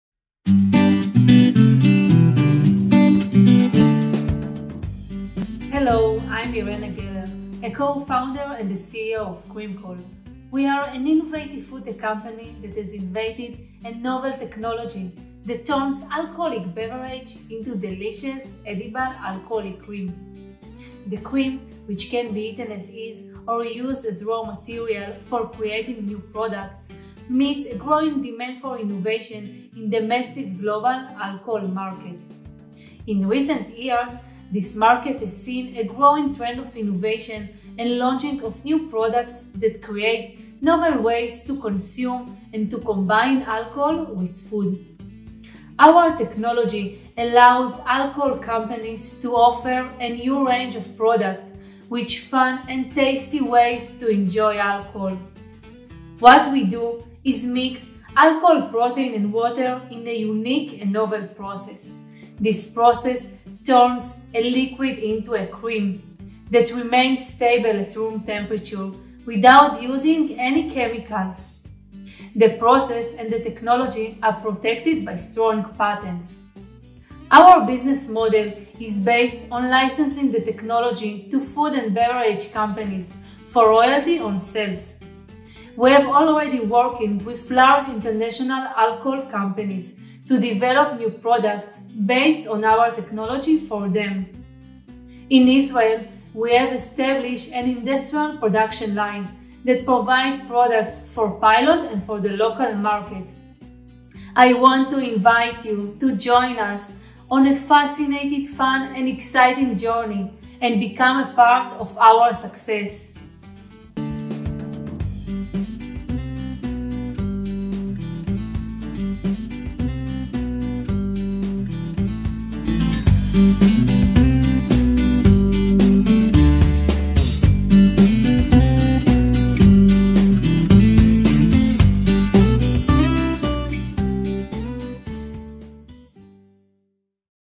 Meet Irena Geller, CreamCol's CEO
Meet Irena Geller, a co-founder and the CEO of CreamCol, an innovative FoodTech company that has invented a novel technology, that turns alcoholic beverages into edible alcoholic creams.